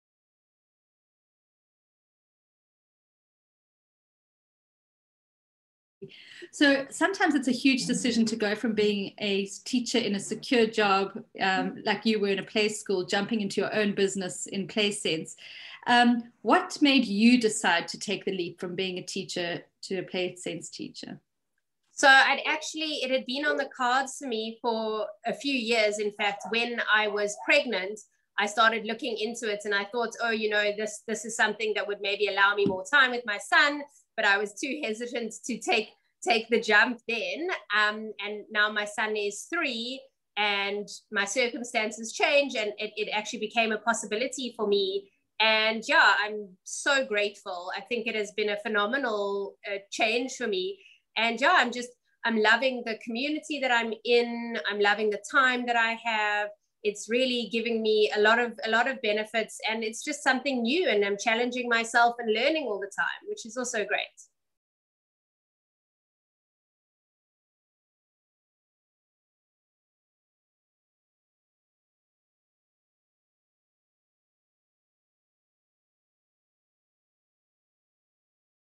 Why I Love Play Sense - Adina
Adina explains her love of the Play Sense franchise